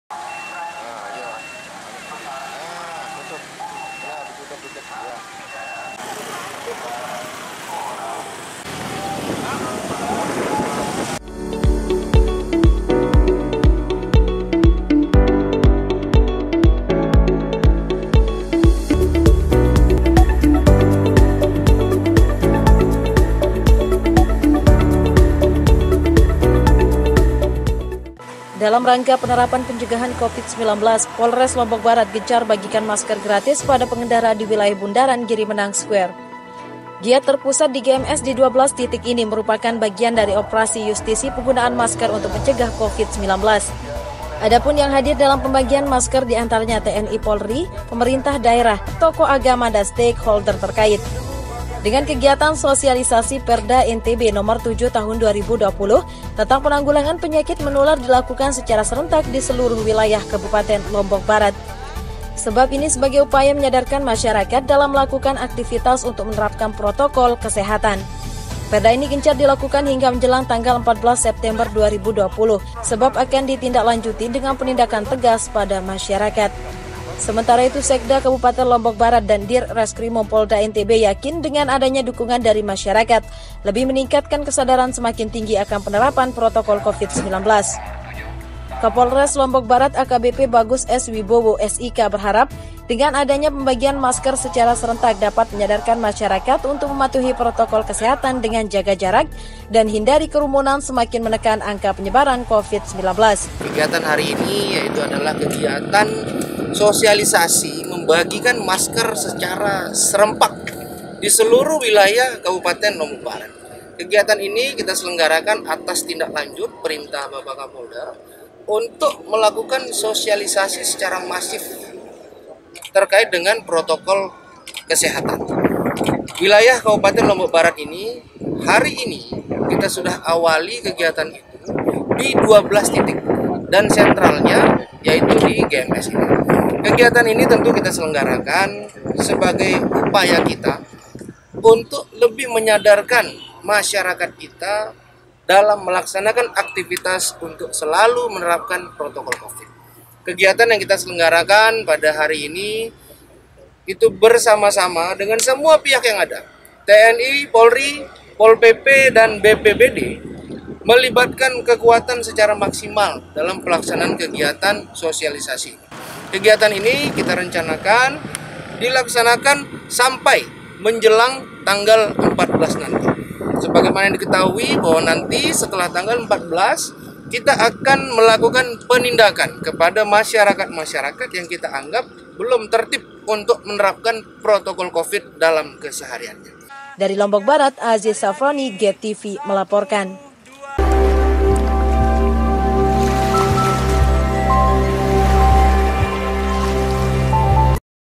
Polres Lobar Gencar Bagikan Masker Gratis Cegah Penyebaran Covid 19
Dalam rangka penerapan pencegahan Covid-19, Polres Lobar gencar bagikan masker Gratis pada pengendara di wilayah Bundaran Giri menang Square
Giat Terpusat di GMS di 12 titik ini, merupakan bagian dari Operasi Yustisi Penggunaan Masker untuk mencegah covid - 19.
Adapun yang hadir dalam pembagian masker diantaranya TNI-Polri, pemda, Dir Reskrimum Polda NTB Kapolres Lombok Barat, Sekda Lobar, tokoh agama dan stake holder trkait
Dengan kegiatan Sosialisasi Perda NTB Nomor 7 tahun 2020 tentang penanggulangan penyakit menular di lakukan secara serentak di seluruh  wilayah kabupaten lombok barat.
Sebab ini sebagai upaya  menyadarkan masyarakat dalam melakukan aktivitas  untuk menerapkan protokol Kesehatan
Perda ini gencar dilakukan hingga menjelang tanggal 14 September 2020 sebab akan ditindak lanjuti dengan penindakan Tegas pada masyarakat
Semntara itu Sekda Kabupaten Lombok Barat dan Dir Reskrimum Polda NTB yakin dengan adanya dukungan dari masyarakat lebih meningkat kan kesadaran semakin tinggi akan penerapan protokol Covid-19,
Kapolres Lombok Barat AKBP Bagus S. Wibowo, SIK berharap dengan adanya pembagian masker secara serentak dapat menyadarkan masyarakat untuk menatuhi protokol kesehatan dengan jaga jarak dan hindari kerumunan semakin menekan angka penyebaran Covid-19